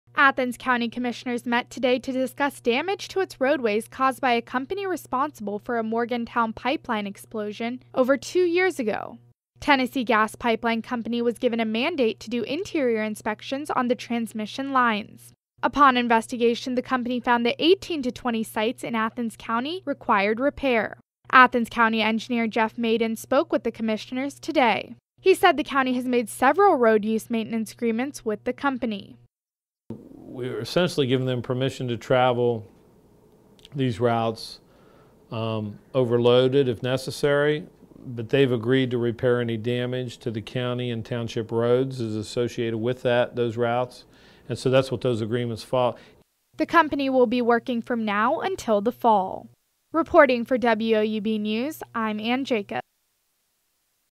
Gas Line Company To Repair Pipes In Athens County
Almost three years since a pipeline explosion in Morgan County, Tennessee Gas Pipeline company will be making repairs in Athens County. WOUB's Ann Jacob reports.